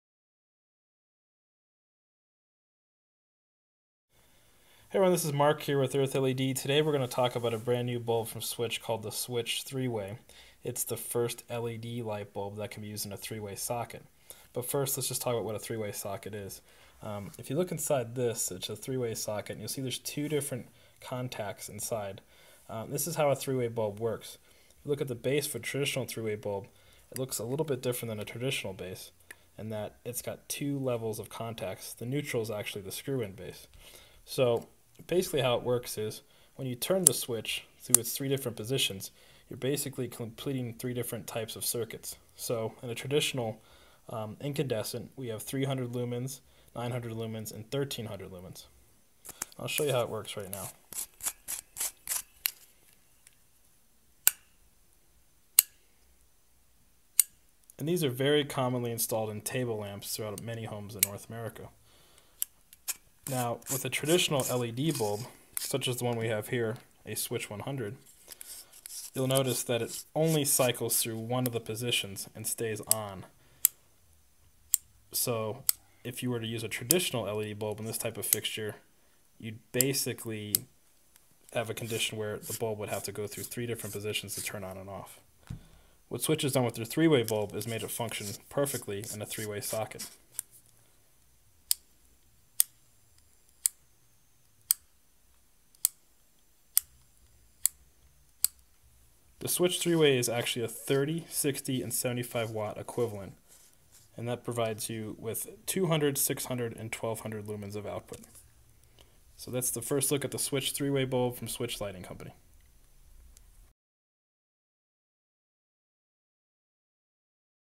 SWITCH3-Way First Look: World's First Three-Way LED Light Bulb
EarthLED reviews the SWITCH3-way LED Bulb from SWITCH Lighting Company, the world's first LED light bulb that works with three way sockets and light switches.